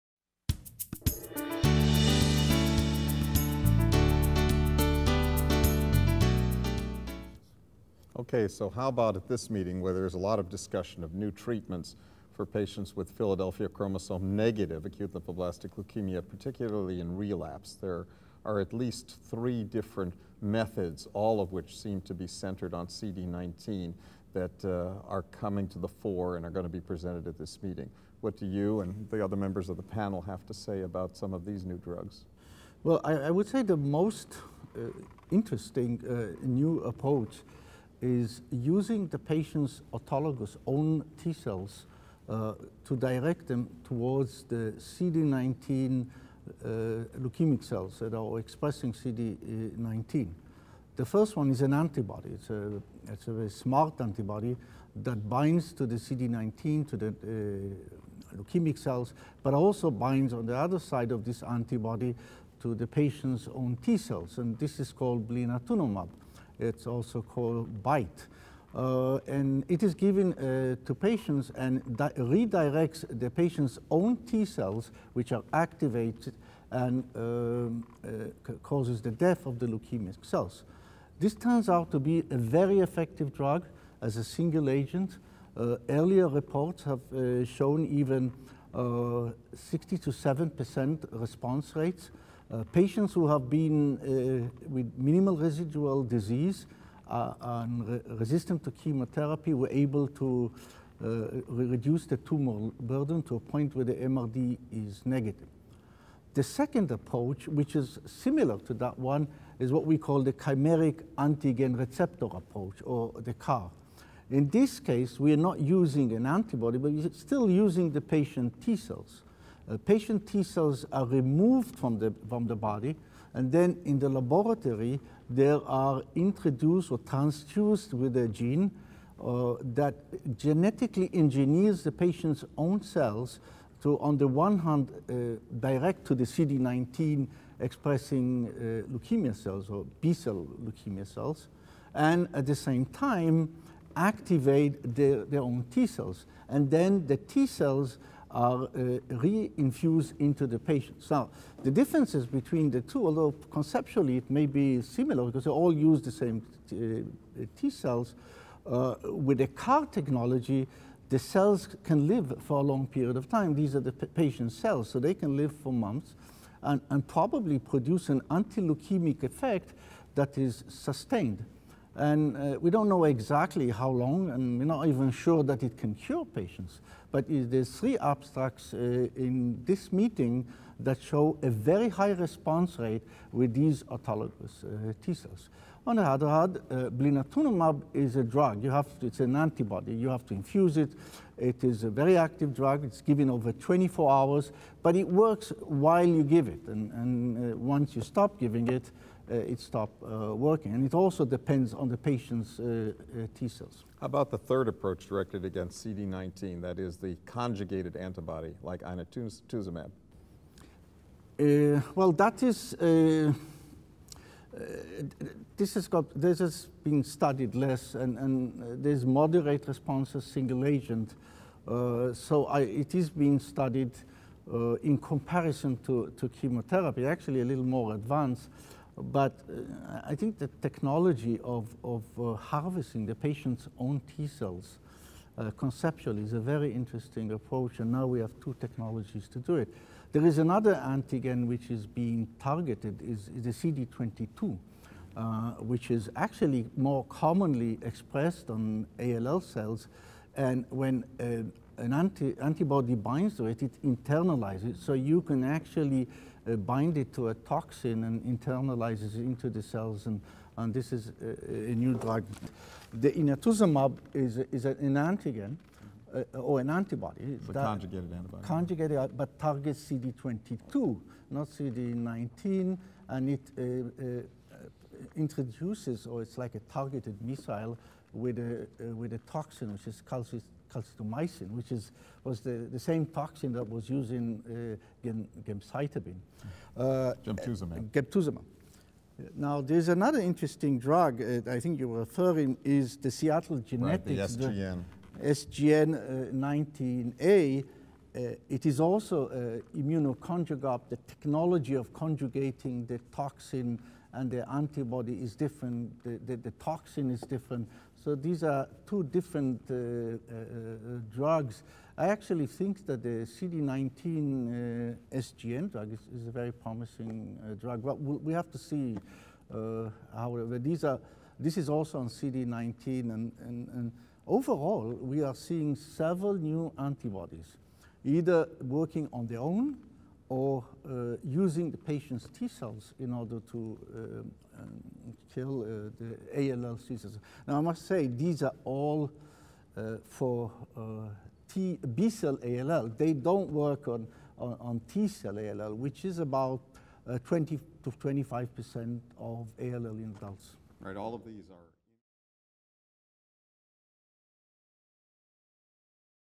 Novel Therapies for Patients Ph-Negative ALL
There are currently several novel therapies being explored for the treatment of patients with Philadelphia chromosome (Ph)-negative acute lymphoblastic leukemia (ALL), particularly in the relapsed/refractory setting.